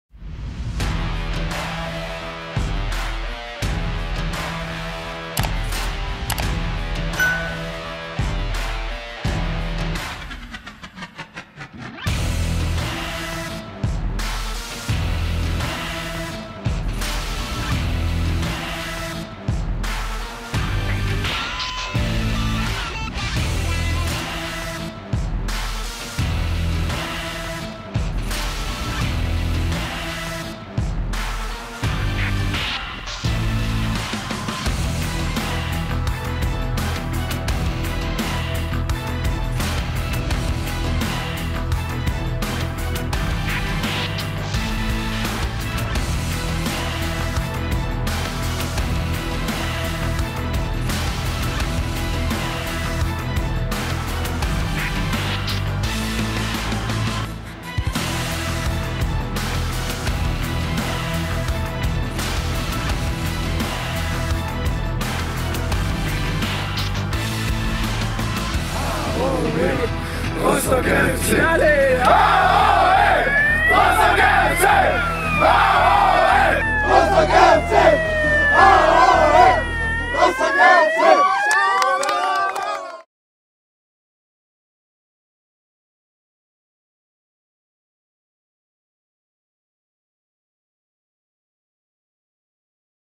Oberliga Action by Rostocker FC
Der RostockerFC ist in seiner zweiten Saison der NOFV Oberliga.

Als Medienpartner für die Videoberichte haben wir aus dem entstandenen Material einen kleinen Action-Trailer zusammen geschnitten.